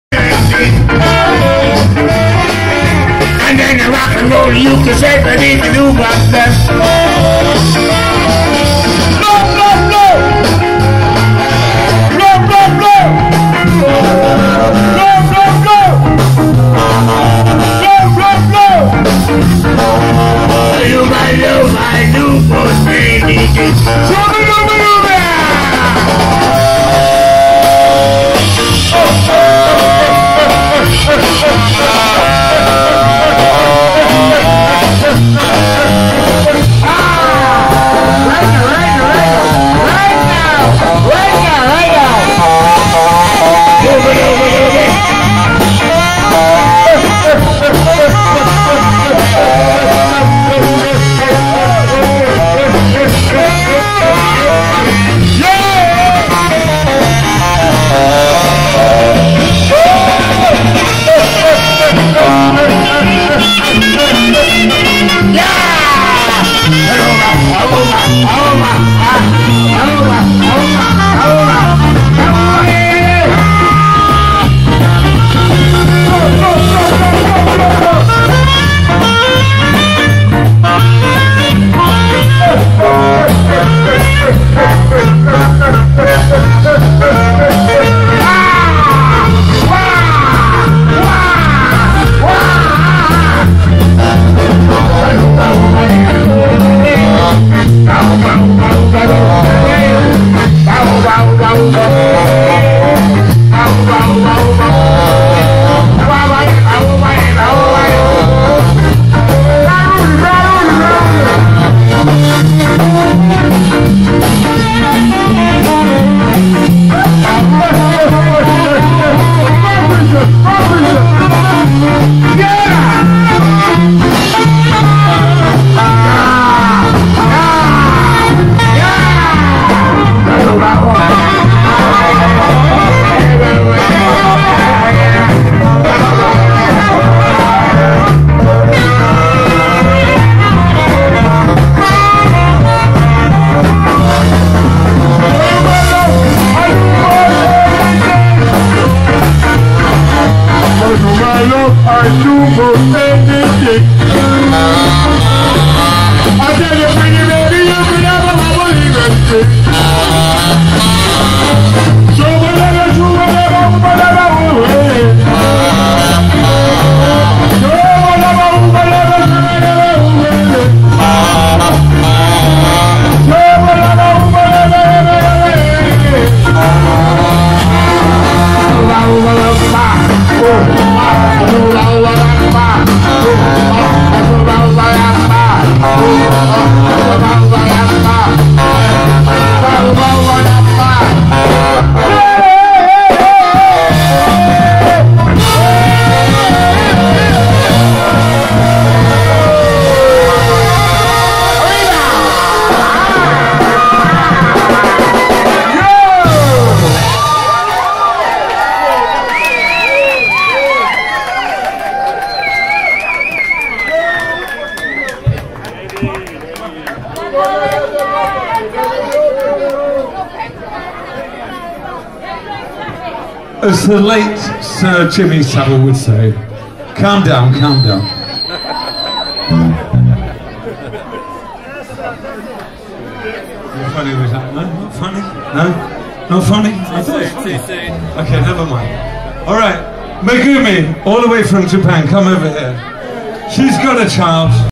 Gaz Mayall & The Trojans
Gaz's 55th Birthday Party Under the Westway 29th Dec 2012